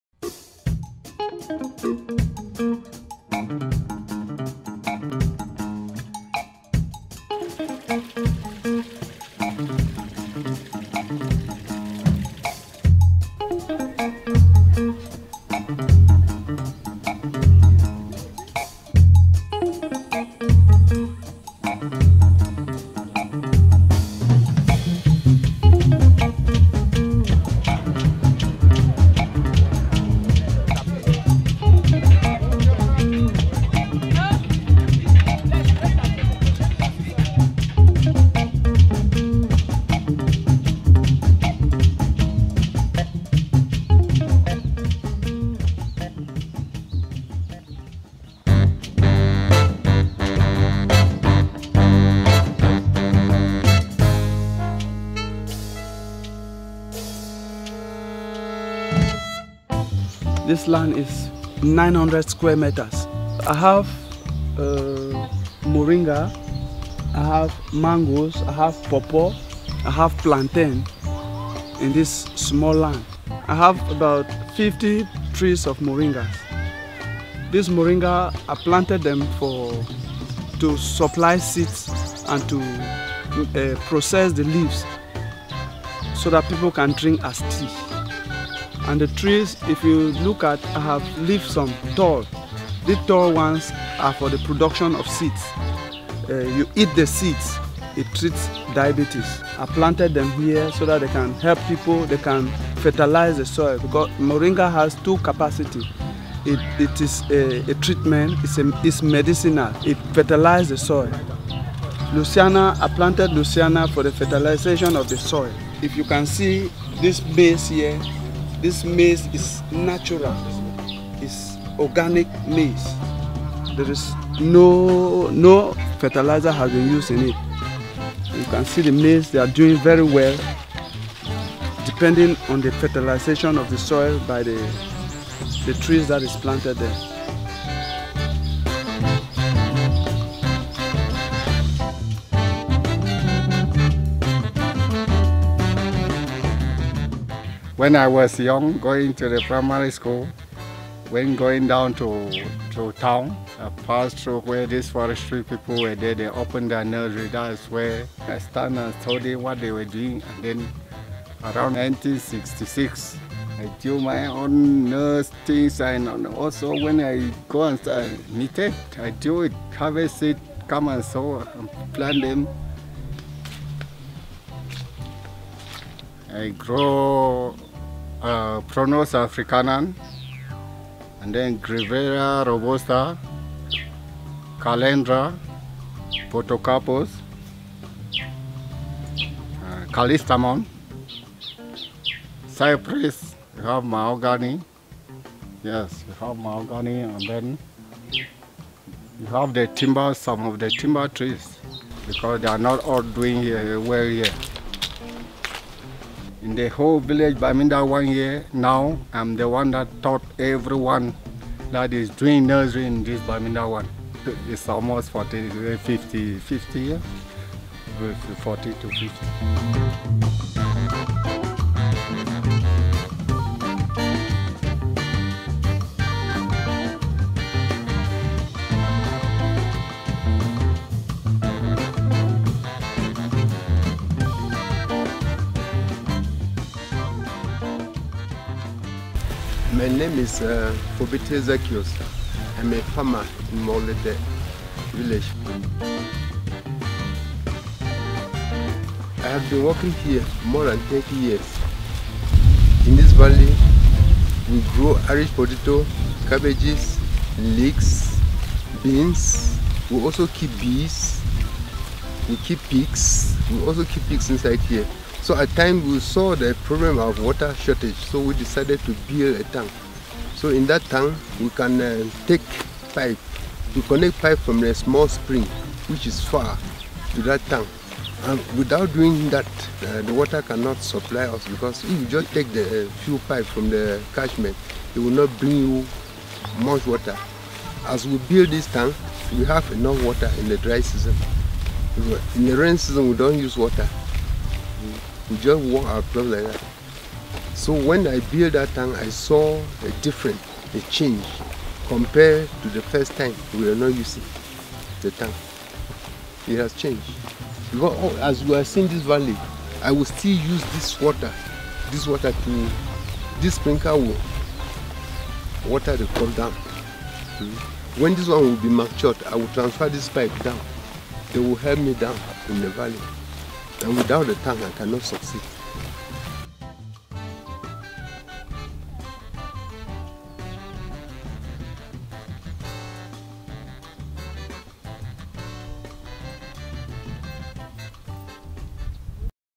Forward-Thinking Farmers
A visit to three different farms in Cameroon provides an overview of the conservation initiatives taking place there. Agroforestry enables Ayuk Rudolf to plant his land with a variety of nitrogen-fixing trees, some of which are medicinal. They also improve the soil for his organic corn and fruit trees. Francis Nfongang has been propagating trees at his nursery for nearly 50 years. After passing the forest service on his way to primary school every day, he was inspired to make his own nursery and now he teaches others how to sow seeds. Forbetiah Zaccheous, after constructing a holding tank for water, sees the results this water reserve on his crops during the dry season. These three stories provide some insight into how, by changing methods and thinking holistically, the land's productivity is improved while benefiting the environment as well.